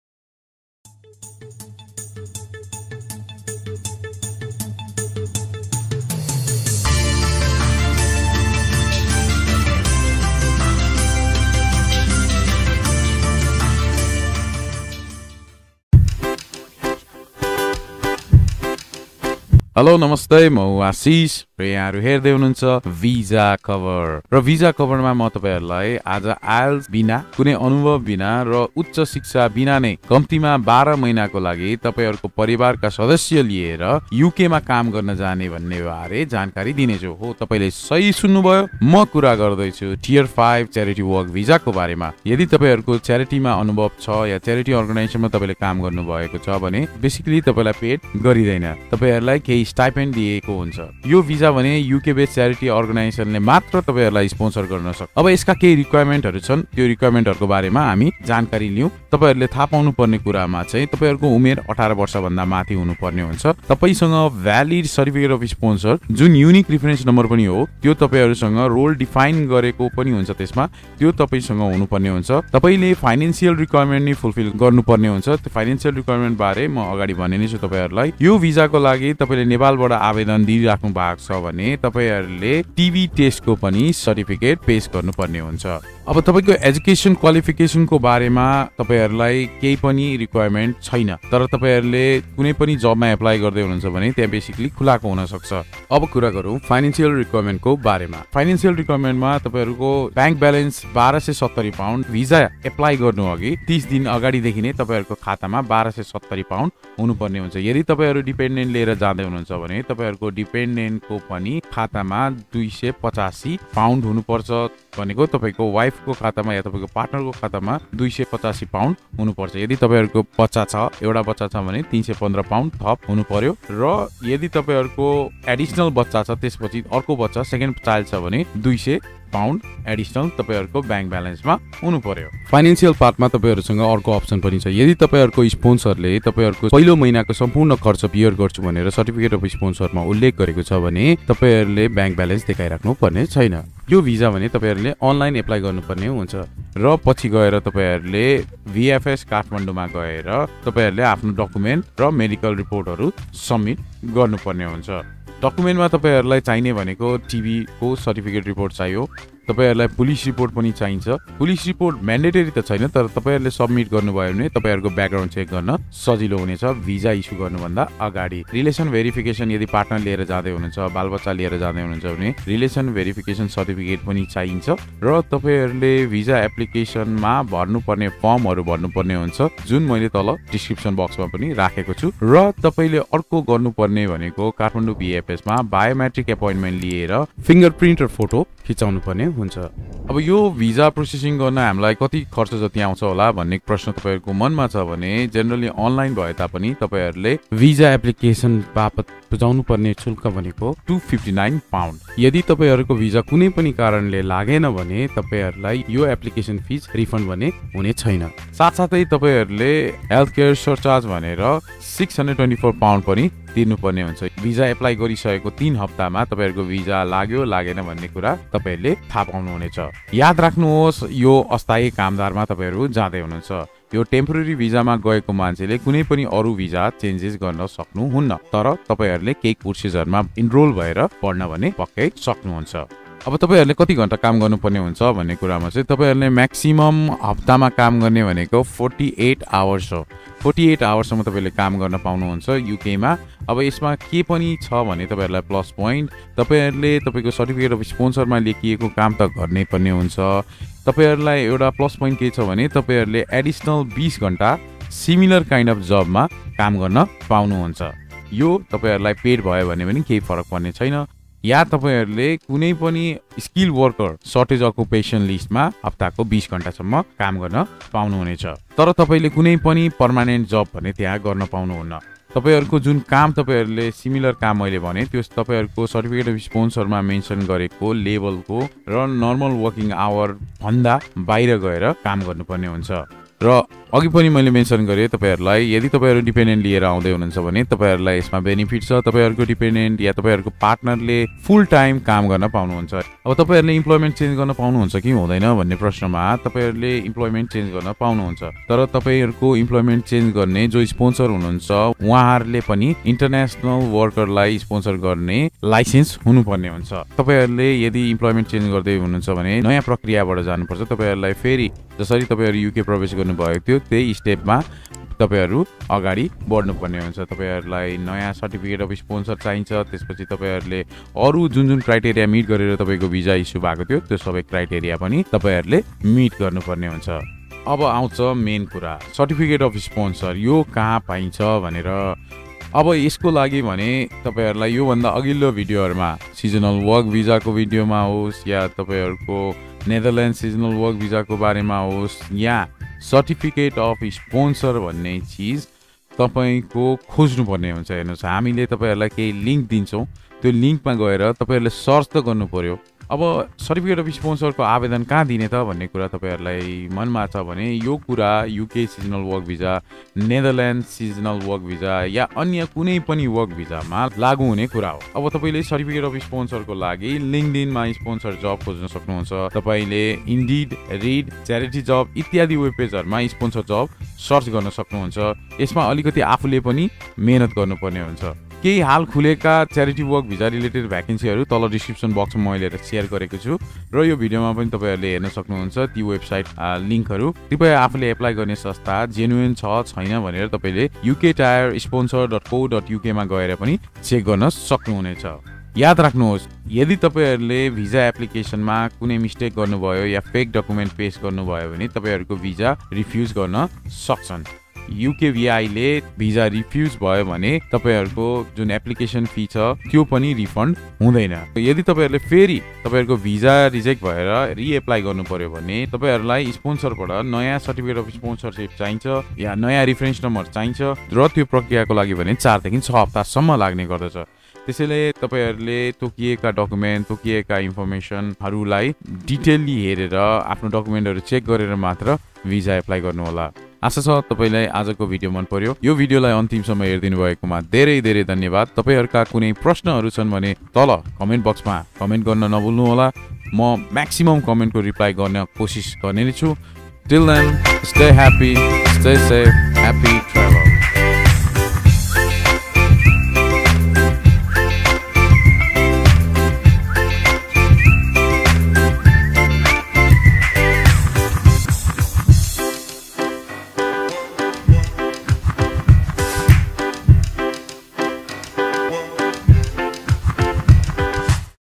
A Complete Guide To United Kingdom Charity Work Visa | VisaKhabar
The Charity Worker Visa route is for individuals who have a job offer to undertake voluntary work in the UK for up to 12 months. The Charity Worker Visa is not a route to settlement, but a partner and children under the age of 18 can apply as dependents on this route.

In order to qualify as charity work under the Charity Worker Visa, the work you are planning to undertake must meet three criteria.

First, it must be voluntary fieldwork that directly contributes to your sponsor’s charitable purpose. This includes activities that would not normally be offered for a wage or salary. It does not include supportive work such as routine administrative, retail, or fund-raising roles.

Second, the work cannot be paid or remunerated, unless it is for reasonable expenses incurred in order to perform your duties.

Finally, you must not be filling a position (even temporarily) that is required on a permanent basis.

Further, in this video we will let you know about the Temporary Work (T5) – Charity Worker visa and sponsored jobs you can apply for now. The links mentioned in the video have been shared for reference and informational purposes only.

LINK TO THE CONTENT MENTION IN THE VIDEOS

Organisations licensed to sponsor workers on the Worker and Temporary Worker immigration routes